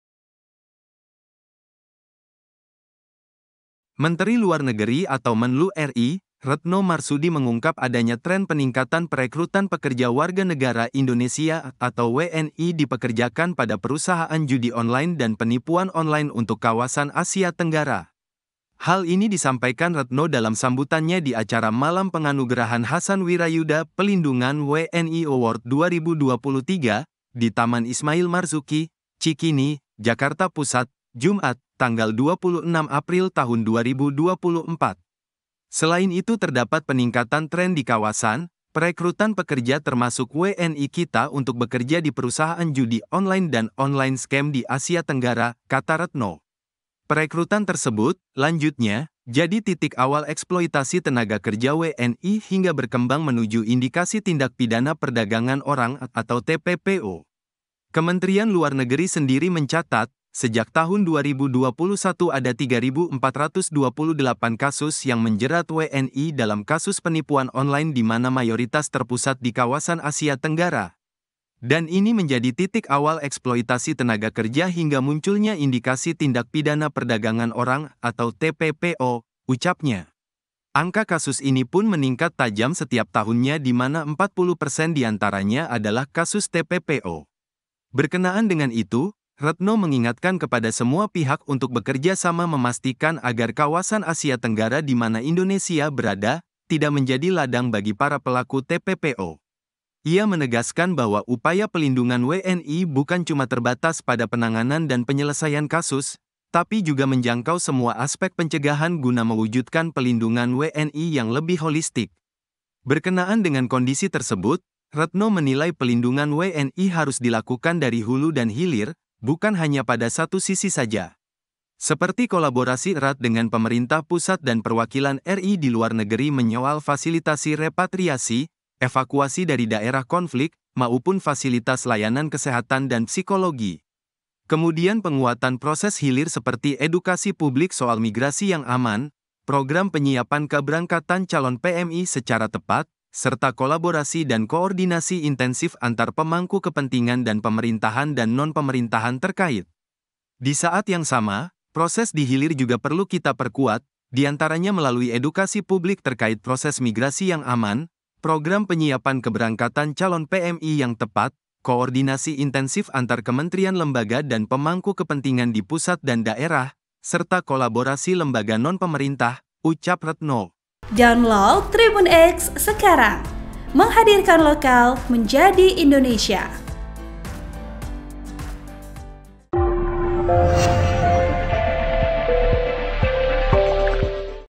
Tanggapan Menlu RI Tentang Tren WNI Direkrut jadi Admin Judi & Penipuan Online di Area Asia Tenggara
POS BELITUNG - Menteri Luar Negeri (Menlu) RI, Retno Marsudi mengungkap adanya tren peningkatan perekrutan pekerja warga negara Indonesia (WNI) dipekerjakan pada perusahaan judi online dan penipuan online untuk kawasan Asia. Tenggara.

Hal ini disampaikan Retno dalam sambutannya di acara Malam Penganugerahan Hassan Wirajuda Pelindungan WNI Award 2023, di Taman Ismail Marzuki (TIM), Cikini, Jakarta Pusat, Jumat (26/4/2024).

"Selain itu terdapat peningkatan tren di kawasan, perekrutan pekerja termasuk WNI kita untuk bekerja di perusahaan judi online dan online scam di Asia Tenggara," kata Retno.

Perekrutan tersebut, lanjutnya, jadi titik awal eksploitasi tenaga kerja WNI hingga berkembang menuju indikasi tindak pidana perdagangan orang atau TPPO.

Kementerian Luar Negeri sendiri mencatat, sejak tahun 2021 ada 3.428 kasus yang menjerat WNI dalam kasus penipuan online di mana mayoritas terpusat di kawasan Asia Tenggara.

"Dan ini menjadi titik awal eksploitasi tenaga kerja hingga munculnya indikasi tindak pidana perdagangan orang atau TPPO," ucapnya.

Angka kasus ini pun meningkat tajam setiap tahunnya di mana 40 persen diantaranya adalah kasus TPPO.

Berkenaan dengan itu, Retno mengingatkan kepada semua pihak untuk bekerja sama memastikan agar kawasan Asia Tenggara di mana Indonesia berada, tidak menjadi ladang bagi para pelaku TPPO.

Ia menegaskan bahwa upaya pelindungan WNI bukan cuma terbatas pada penanganan dan penyelesaian kasus, tapi juga menjangkau semua aspek pencegahan guna mewujudkan pelindungan WNI yang lebih holistik.

Berkenaan dengan kondisi tersebut, Retno menilai pelindungan WNI harus dilakukan dari hulu dan hilir, bukan hanya pada satu sisi saja.

Seperti kolaborasi erat dengan pemerintah pusat dan perwakilan RI di luar negeri menyoal fasilitasi repatriasi, evakuasi dari daerah konflik, maupun fasilitas layanan kesehatan dan psikologi.

Kemudian penguatan proses hilir seperti edukasi publik soal migrasi yang aman, program penyiapan keebrangkatan calon PMI secara tepat, serta kolaborasi dan koordinasi intensif antar pemangku kepentingan dan pemerintahan dan non pemerintahan terkait.

"Di saat yang sama, proses dihilir juga perlu kita perkuat, diantaranya melalui edukasi publik terkait proses migrasi yang aman, program penyiapan keberangkatan calon PMI yang tepat, koordinasi intensif antar kementerian lembaga dan pemangku kepentingan di pusat dan daerah, serta kolaborasi lembaga non-pemerintah," ucap Retno. (*)

(Dwiki)